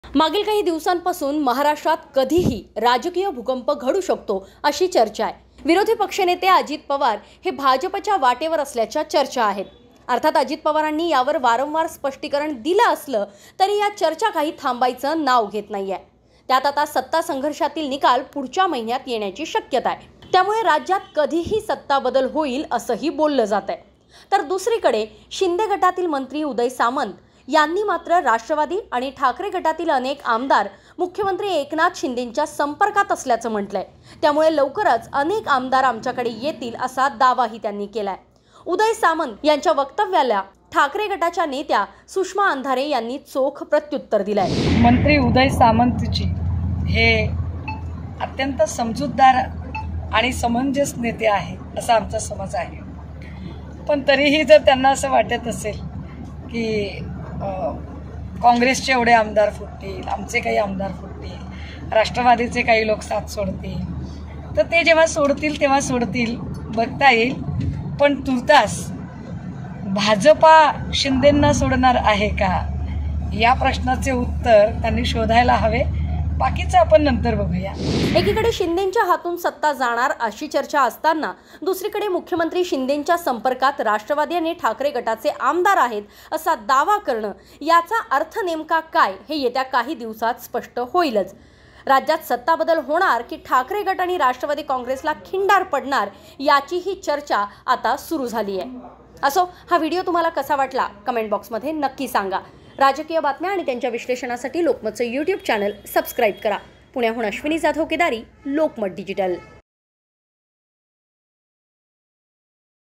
उदय सामंतांच्या दाव्यावर सुषमा अंधारेंचं प्रत्युत्तर! Sushma Andhare on Uday Samant | AJ3
दुसरीकडे शिंदे गटातील मंत्री उदय सामंत यांनी मात्र राष्ट्रवादी आणि ठाकरे गटातील अनेक आमदार मुख्यमंत्री एकनाथ शिंदेंच्या संपर्कात असल्याचं म्हटलं आहे, त्यामुळं लवकरच अनेक आमदार आमच्याकडे येतील असा दावा त्यांनी केलाय (Ashwini Jadhav VO)